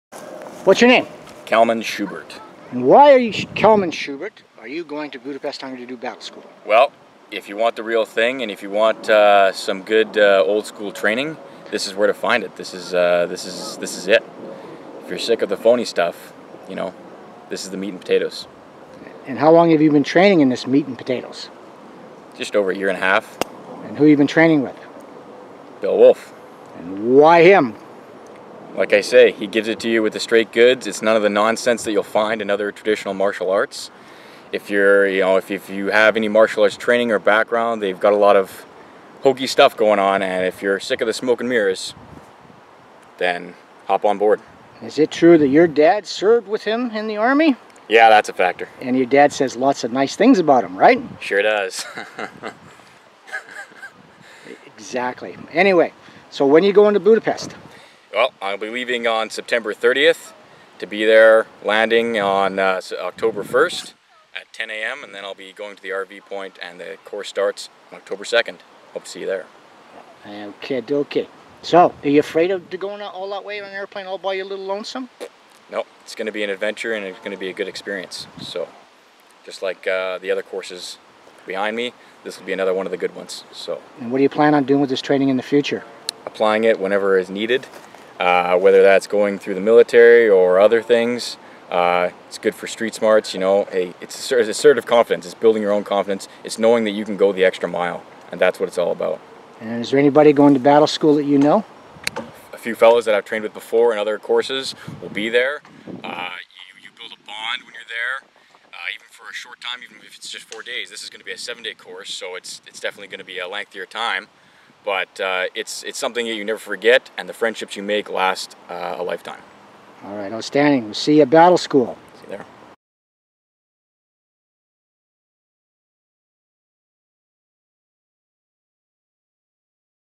BATTLESCHOOL pre-interview | Bill Wolfe | Wolfes Combatives | Defendo
7 days of Military Combatives Training.
Next course October 2-8 2016 . Budapest, Hungary

Bill Wolfe | Founder of Wolfes Combatives / Defendo
Bill Wolfe a Ret. Regimental Sergeant Major CF and a former ERT/SWAT team leader - Canada

Bill Wolfe has been Teaching ‘Civilians’ Defendo / Wolfes Combatives for over 20 years. Wolfe has been instructing Military Combatives and Police Use of Force Tactics for a long time - over 40 years.